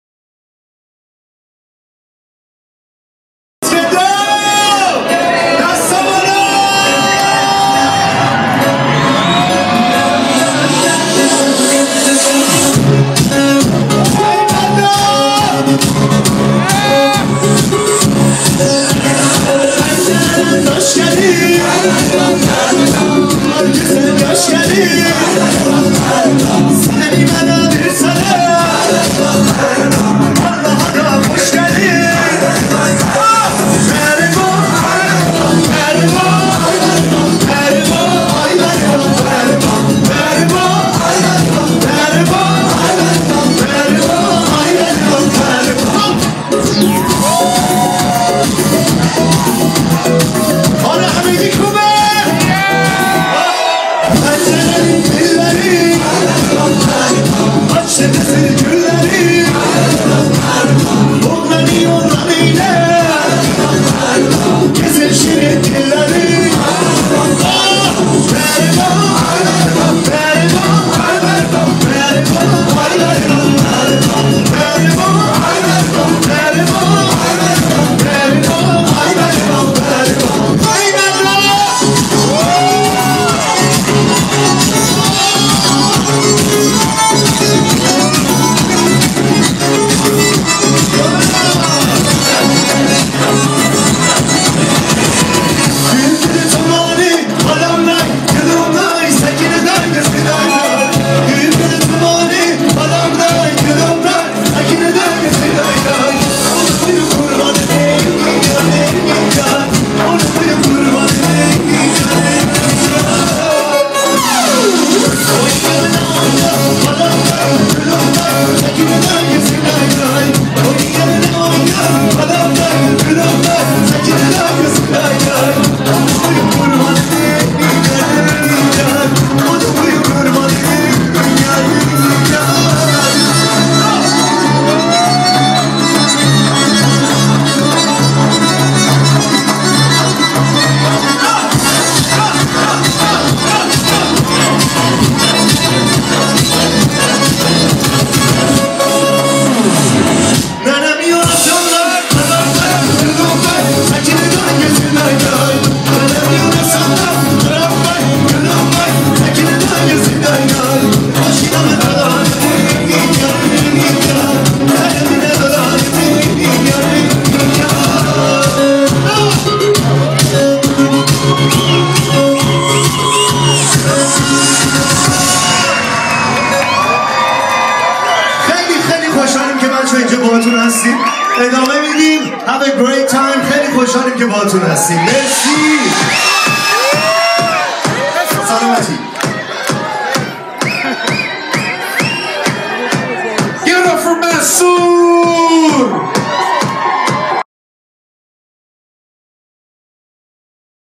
Mansour Las Vegas_Haze dec 2011
Mystery4, Radio Javan, PSAC, Hardcore, Plus One, Xclusive hosted the annual 702 parties held in Las Vegas from Thursday, December 22nd through Sunday, December 25th, 2011. All sold out events with great, beautiful guests, who we all hope had an awesome experience! We have pictures for you from the events at 1OAK (Mirage Hotel), Haze (Aria Hotel), and The Bank (Bellagio Hotel). Music was provided all night by DJs Mohsen, AL, Taba, Alex, and Peyman Z and artists such as Kamran & Hooman, Mansour, Arash, and Sepideh made appearances!